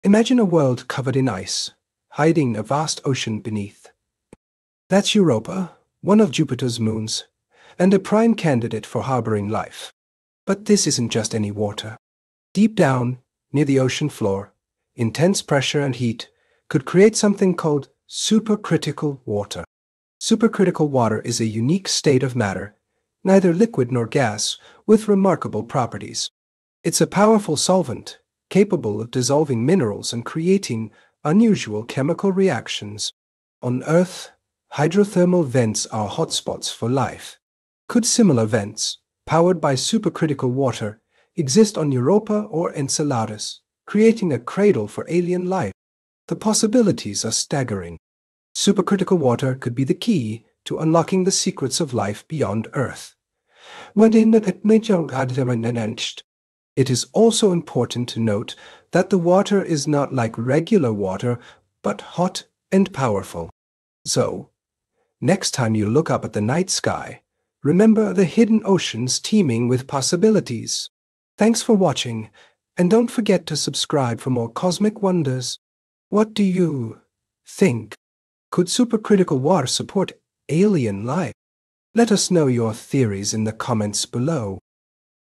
Cosmic Fountains: Supercritical Water on Alien Oceans!
Explore the fascinating possibility of supercritical water and hydrothermal vents on ocean worlds like Europa and Enceladus! Discover the potential for unique chemical reactions and the implications for life beyond Earth.

0:00 Introduction to Europa
0:10 Supercritical Water Explained
0:25 Hydrothermal Vents and Life
0:40 The Potential for Alien Life
0:50 Exploring Ocean Worlds
1:05 Call to Action
1:15 Engage in the comments